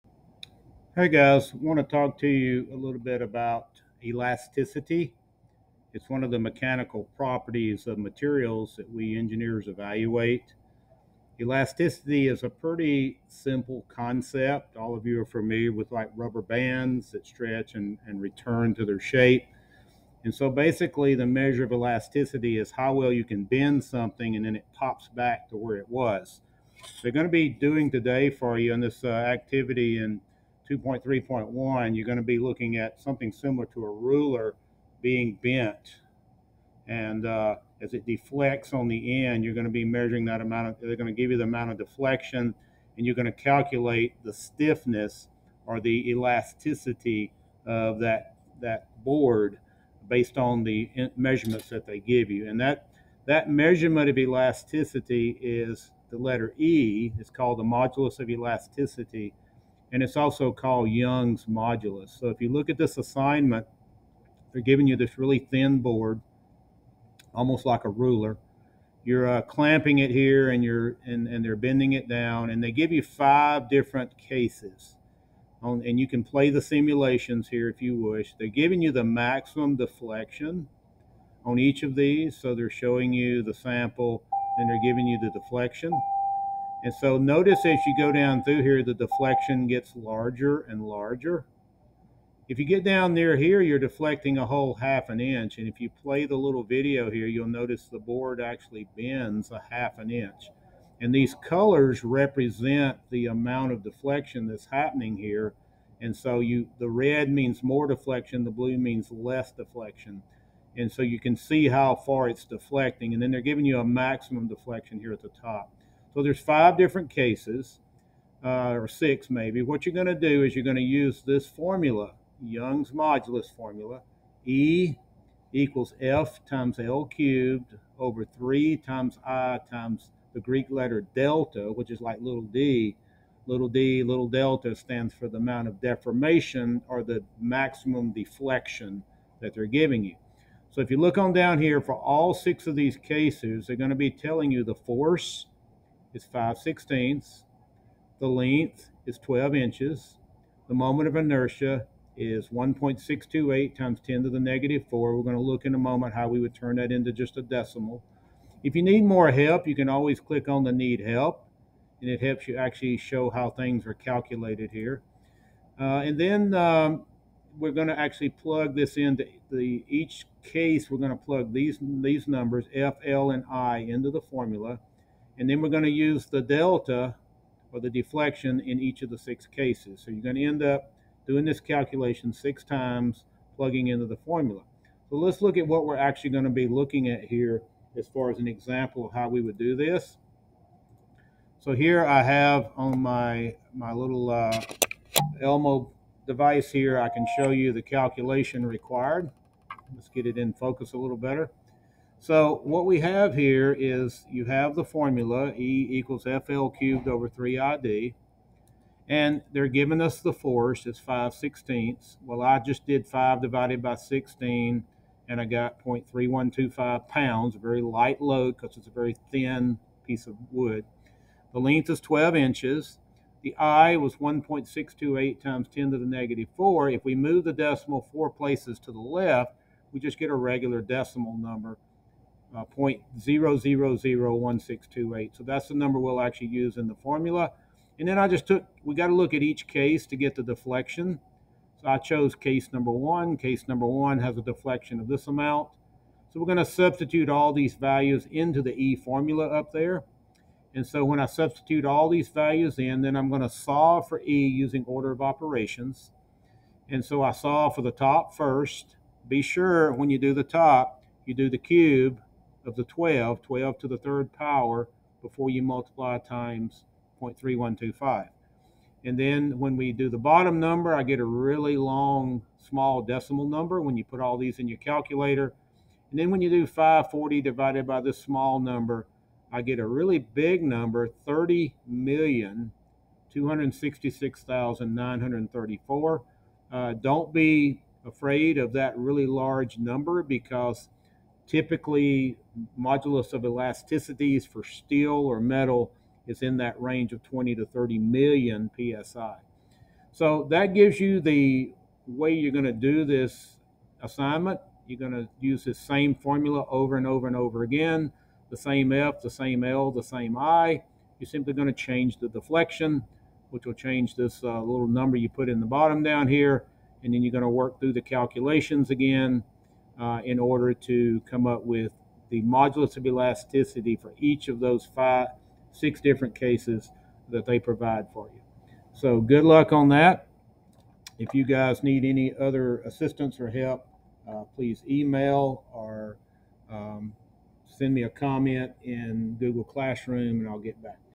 Calculating Young's Modulus of Cantilever Beam
How to apply Young's Modulus formula for a thin cantilever beam.